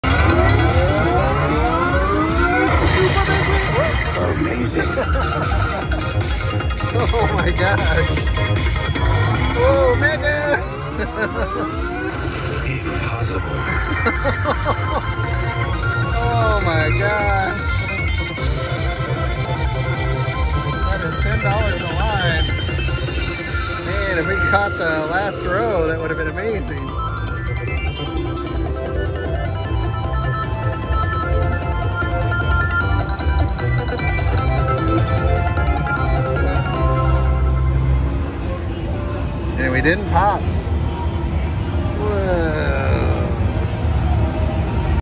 Mega Big Win Great Eagle
Had I landed the last tell it would have been epic! Love landing the ladies and eagle wilds! In the bonus that could have been 25 times the payout!